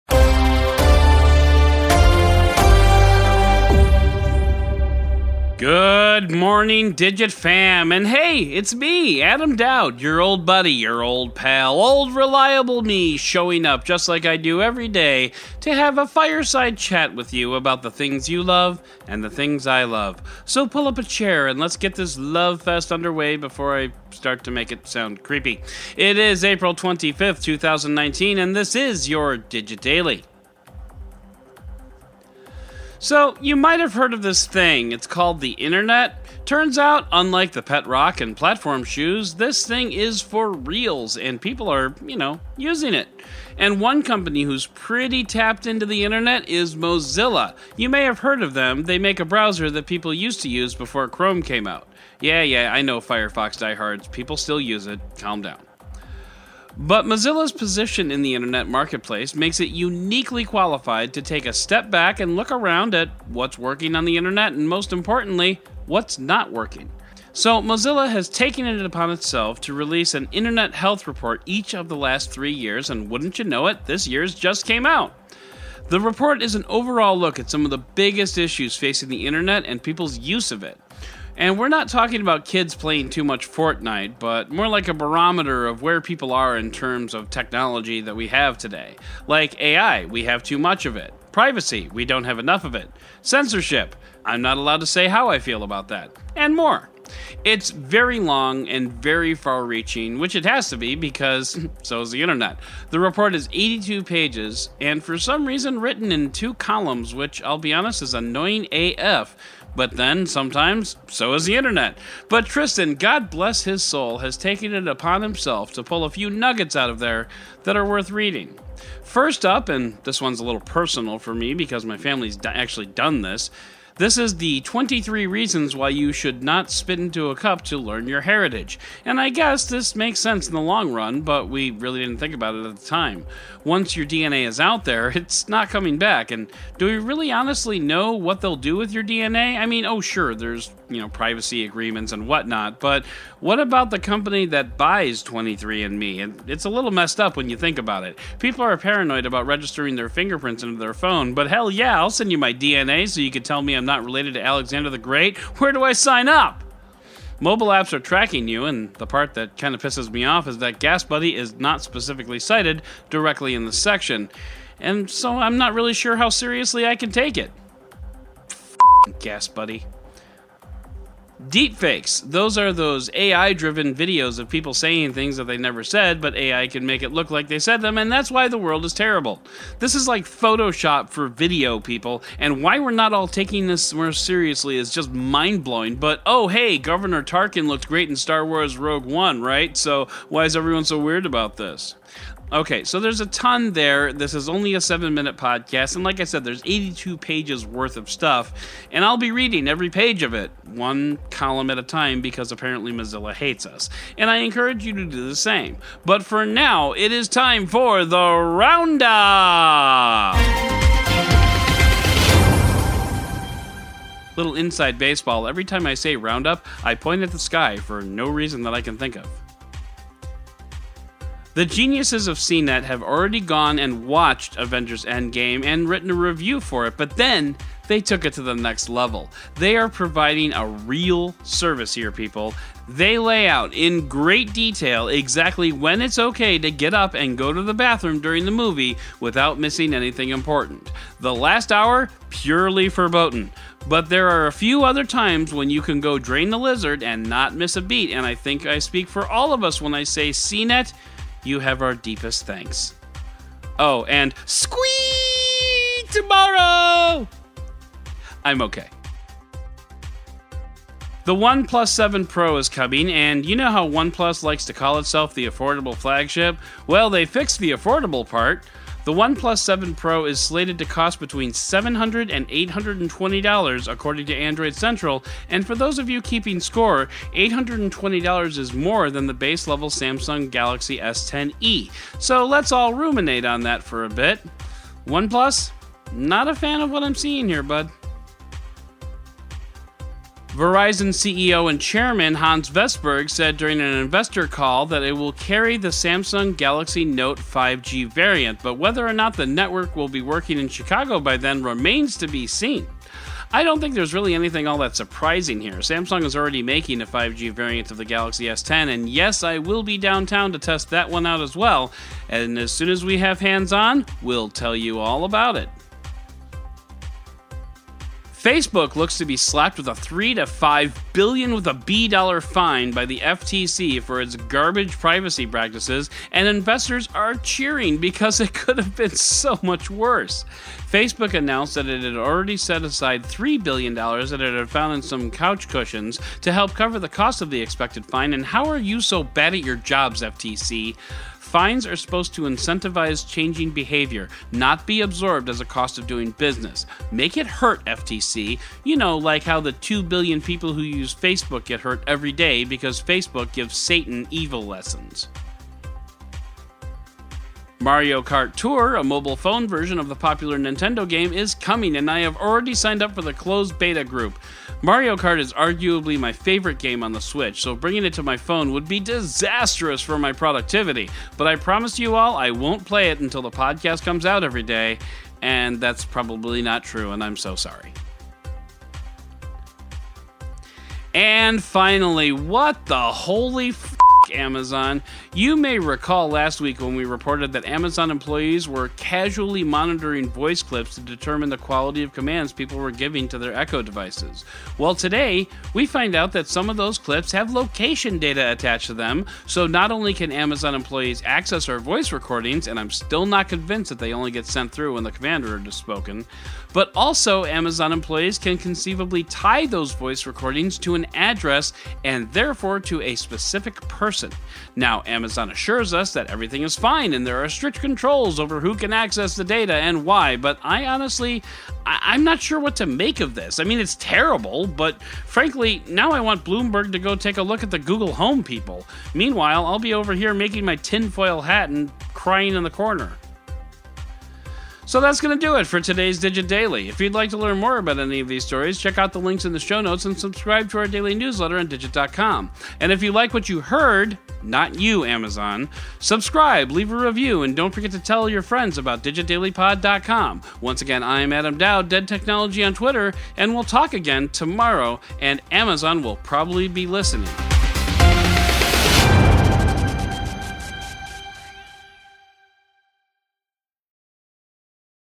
April 25, 2019 - Mozilla’s Internet Health Report
Today Mozilla tells us just how healthy the Internet is, or more accurately how healthy our use of it is.  Spoiler alert:  It’s not so good. From privacy concerns to AI to censorship, there’s a lot that needs to be explored out there.

 Our roundup includes stories about  bathroom breaks during Avengers: Endgame,  pricing for the OnePlus 7 Pro,  Verizon’s Samsung Galaxy Note 5G plans,  Facebook’s three billion dollar slap on the wrist, MarioKart coming to Android, and  if you thought Amazon was bad at privacy, it’s worse than you think.